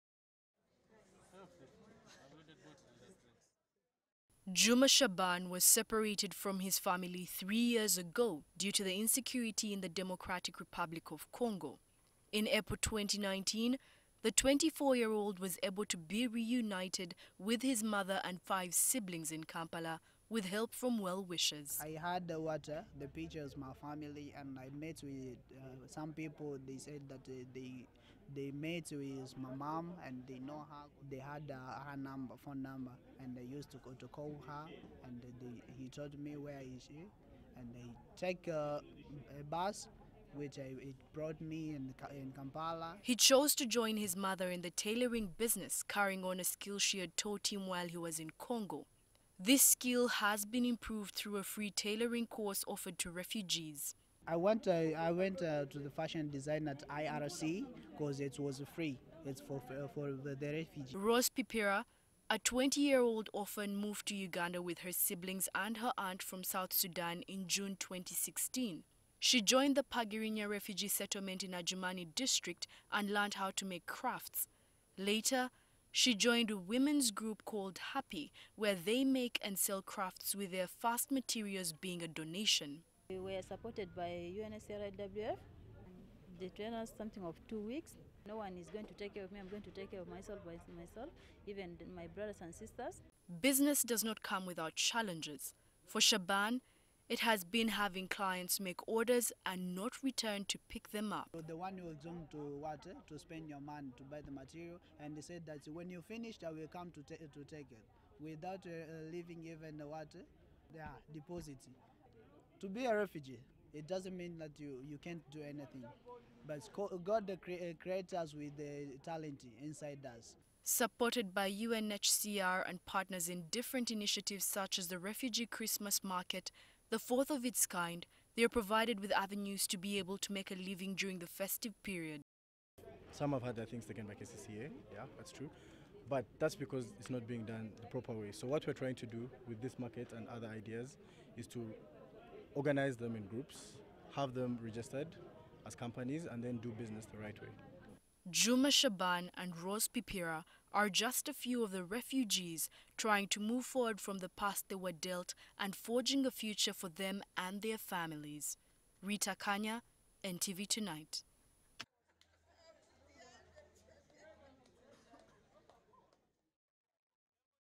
URBAN REFUGEE’S LIFE: Shaban struggles to survive far from home
Uganda hosts over 1.3 million refugees and as a means of coping with the growing numbers, they are encouraged to become self-reliant. The policies in place provide for refugees to work thus enabling them to survive independently without external humanitarian assistance. Rita Kanya shares how some have been able to take up opportunities to earn a living in Uganda.